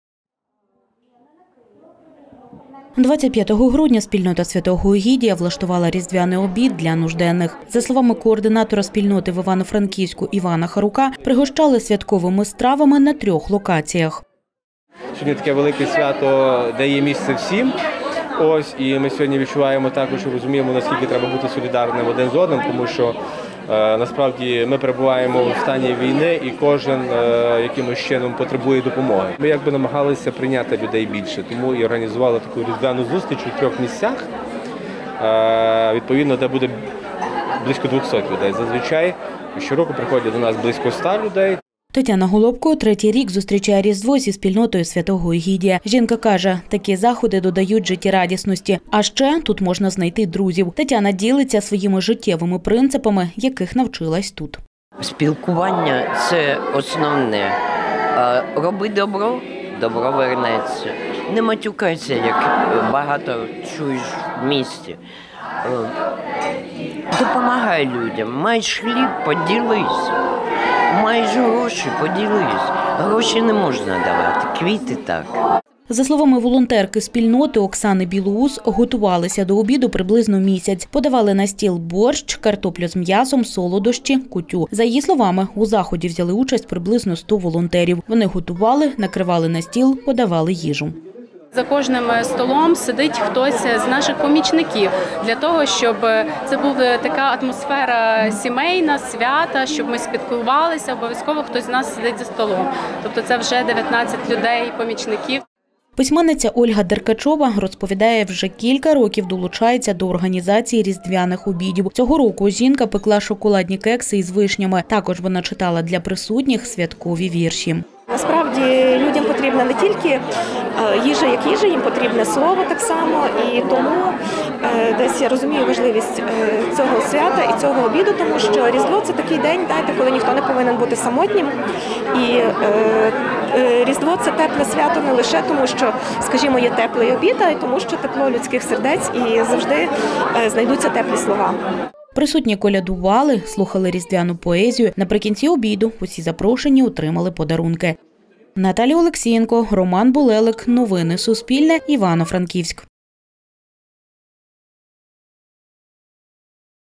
У Франківську влаштували святковий обід для нужденних
В Івано-Франківську волонтери «Спільноти святого Егідія» влаштували 25 грудня різдвяний обід для нужденних. Святковими стравами пригостили приблизно дві сотні людей.